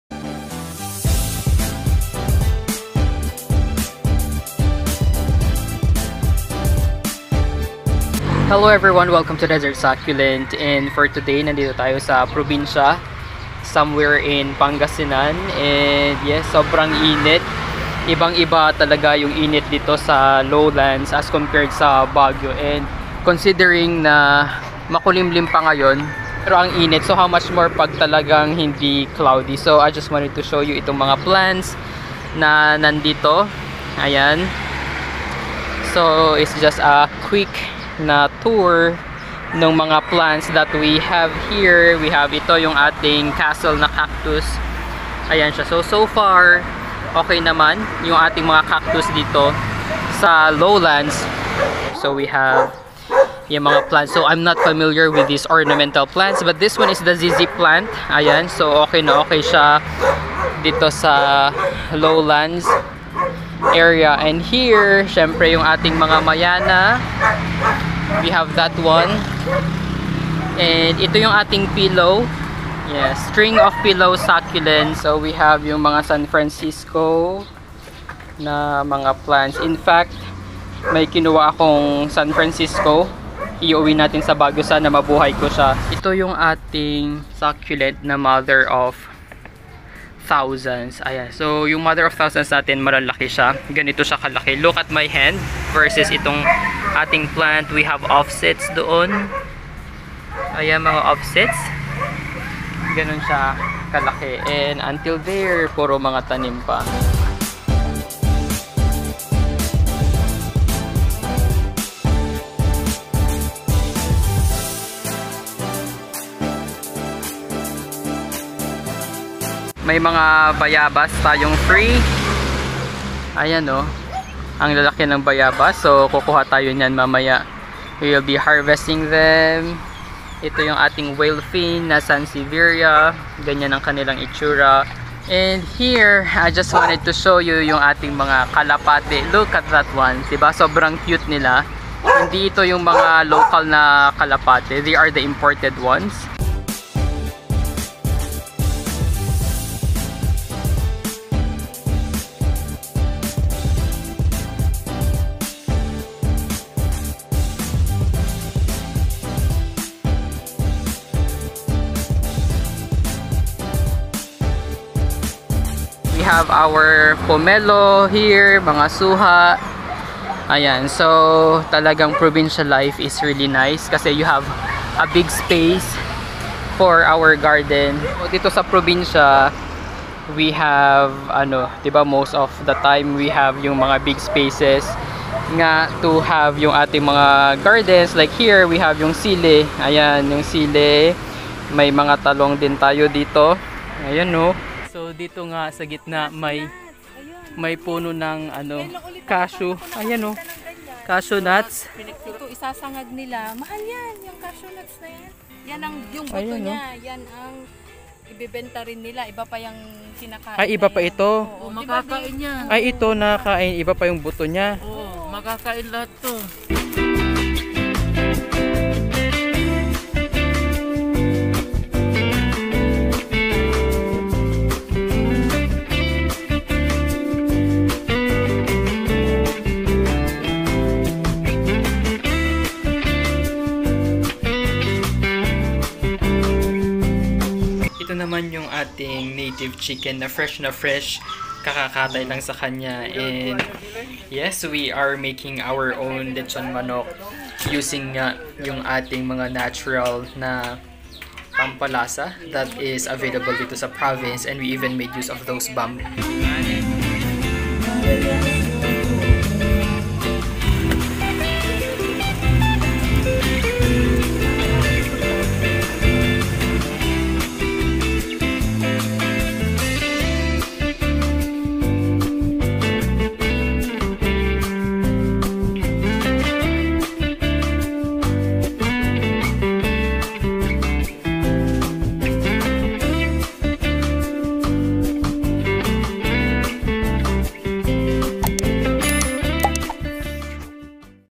Buhay probinsiya Plant tour || Nakapasyal ulit after 1 year of quarantine
Nakavisit din sa wakas sa province. Brings back old memories. 😁